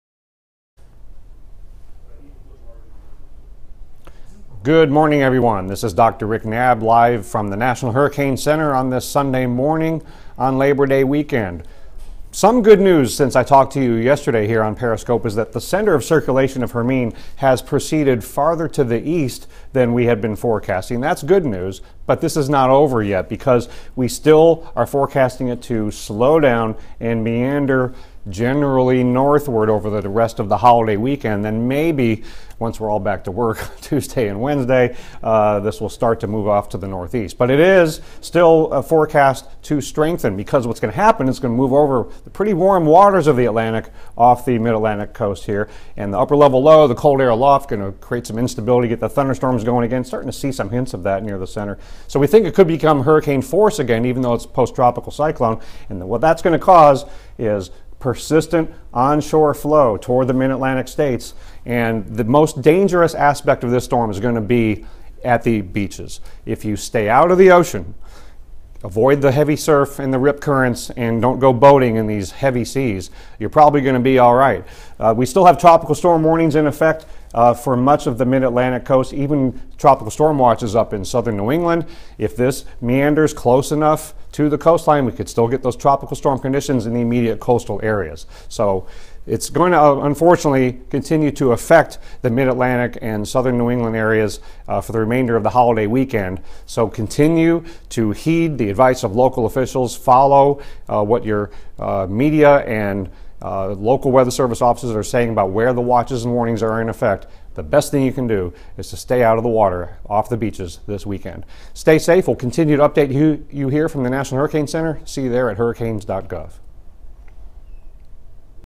NHC Director Dr. Rick Knabb - Post-Tropical Cyclone Hermine
NHC Director Dr. Rick Knabb provides a Periscope briefing on Post-Tropical Cyclone Hermine , Sunday morning, September 4, 2016.

Miami, FL